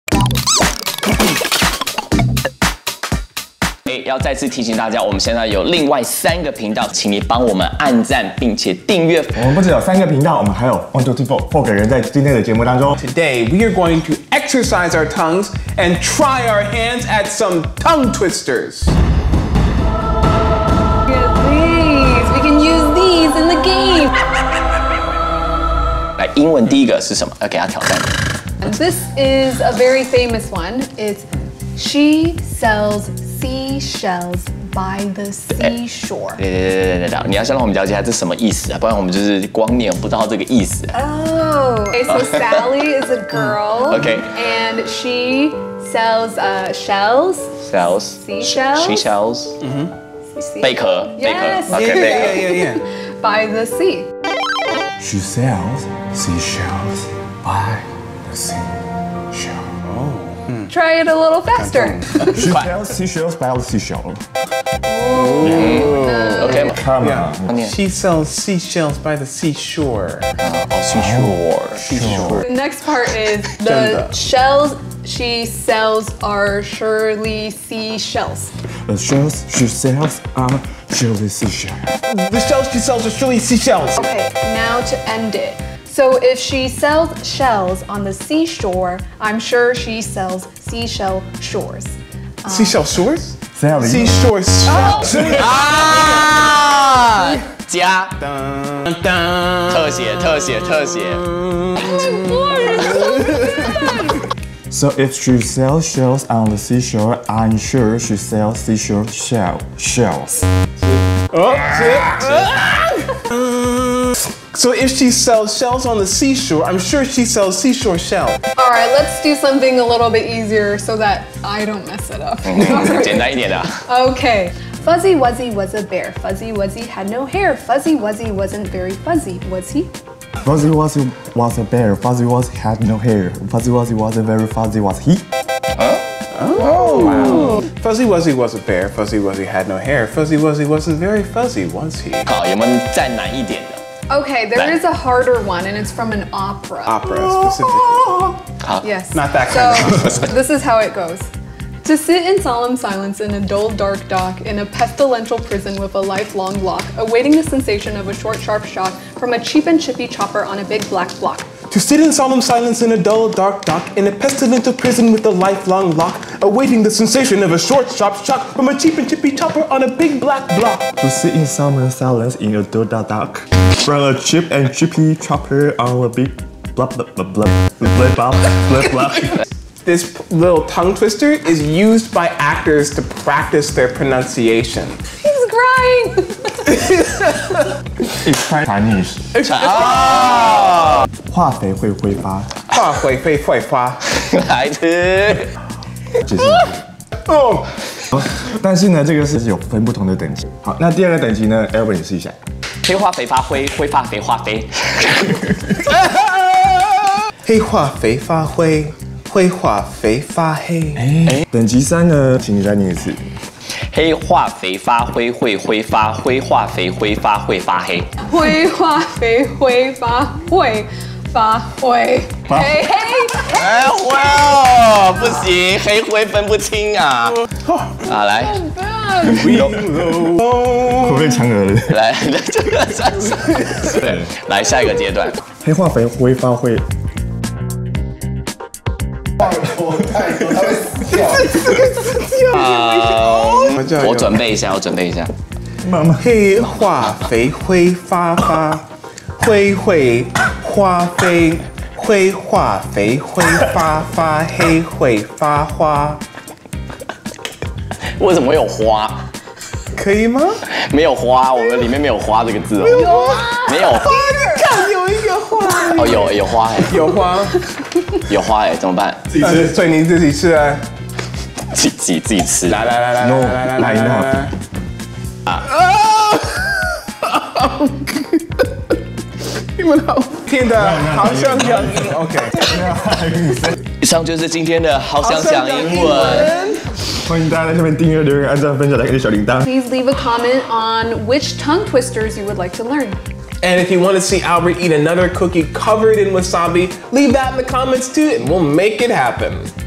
【好想講英文】過年聚在一起玩什麼? 來玩繞口令!
唸到舌頭打結! 快來聽David老師和Manya老師用中文唸繞口令~